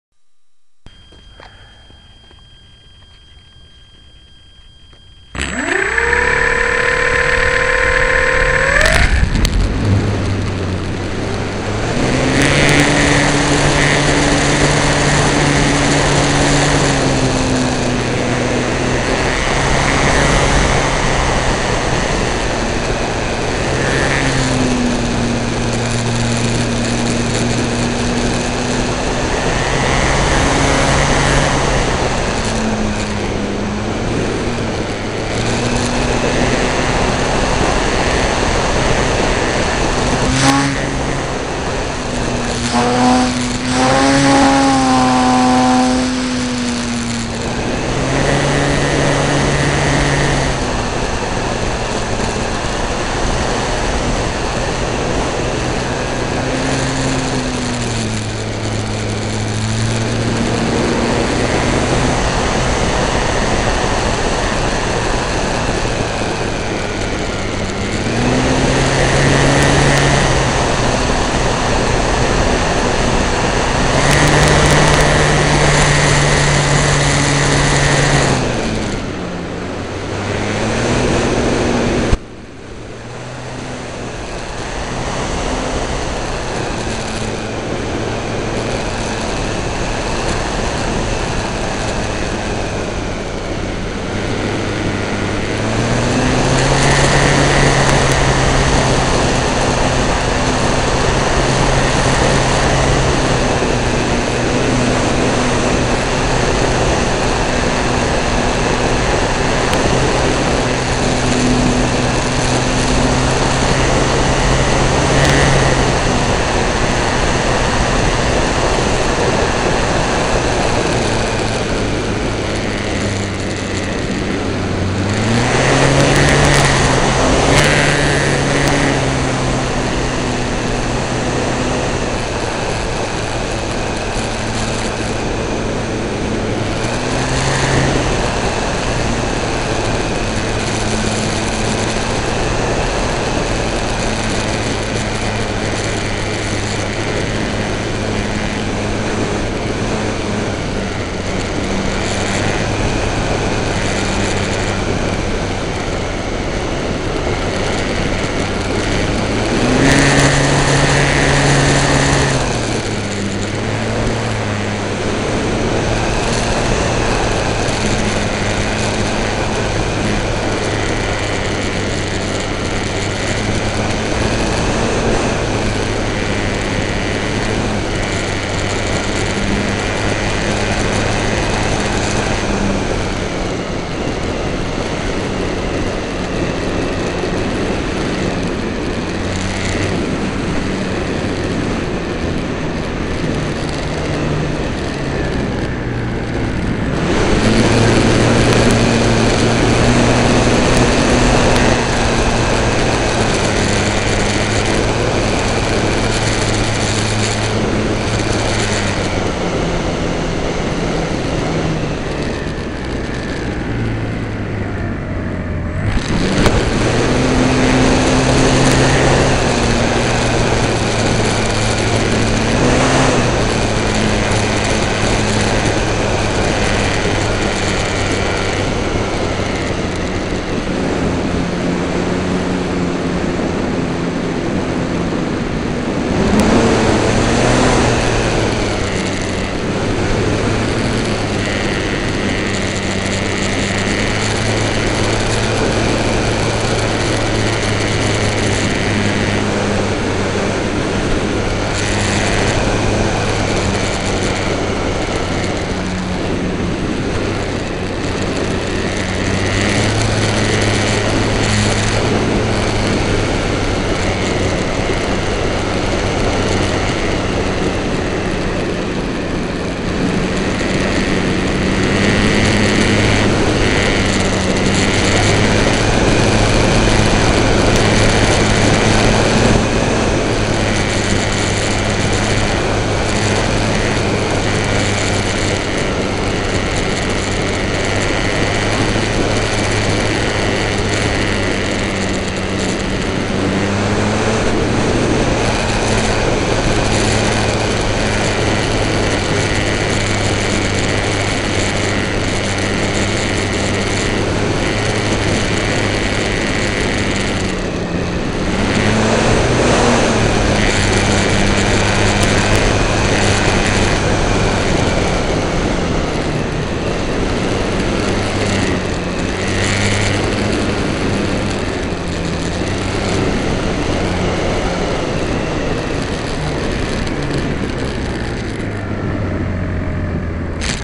Parkzone extra 300 tailcam
Some sport flying with my new PKZ Extra 300, 3rd flight out of the box.  Unbelivably powerful and fast.